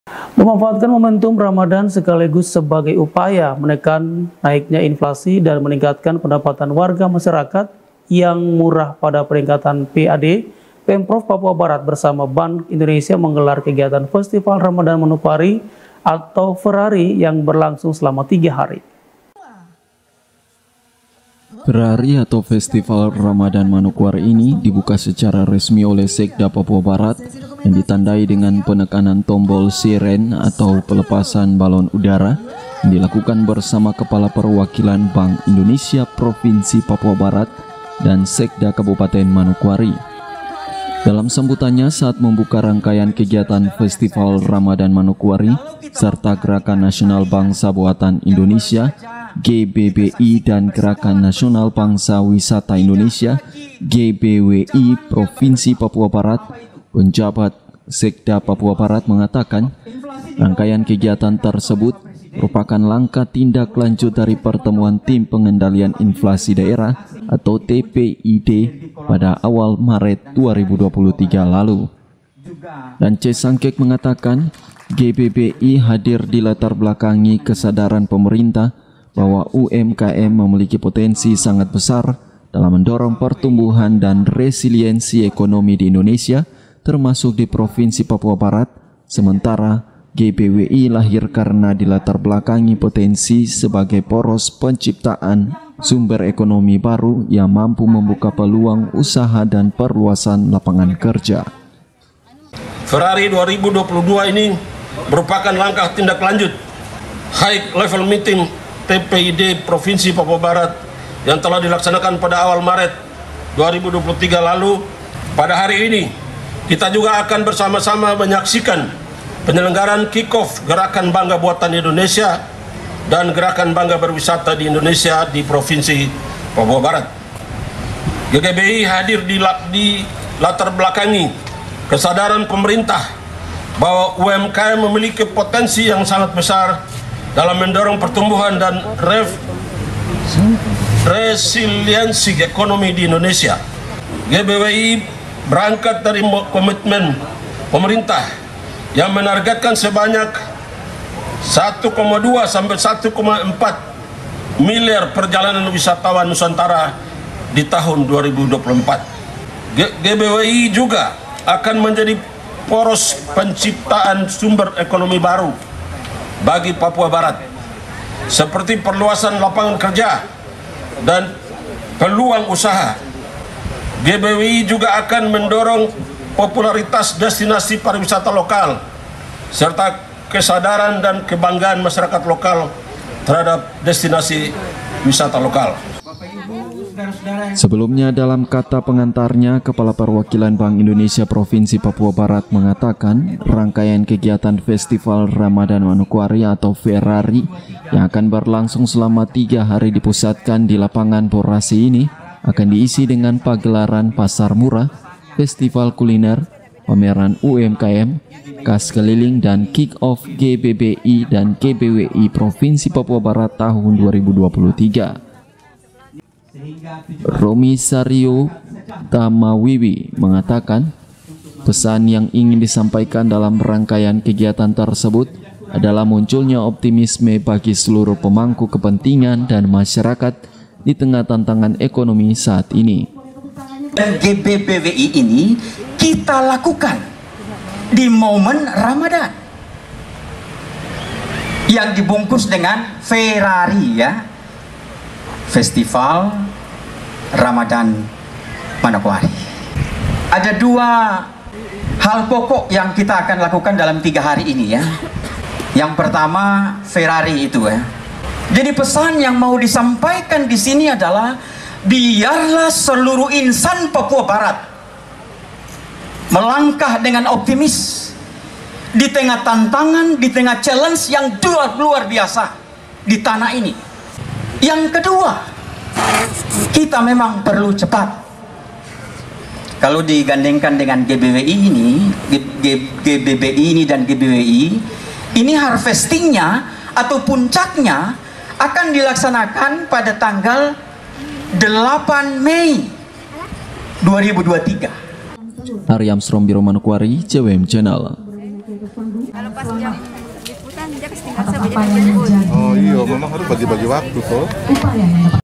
GELARAN FERARI MANOKWARI
MEMANFAATKAN MOMENTUM RAMADHAN SEKALIGUS SEBAGAI UPAYA MENEKAN NAIKNYA INFLASI DAN MENINGKATKAN PENDAPATAN WARGA MASYARAKAT YANG MUARANYA PADA PENINGKATAN PAD PEMPROV PAPUA BARAT BERSAMA BANK INDONESIA MENGGELAR KEGIATAN FESTIVAL RAMADHAN MANOKWARI ATAU FERARI YANG AKAN BERLANGSUNG SELAMA 3 HARI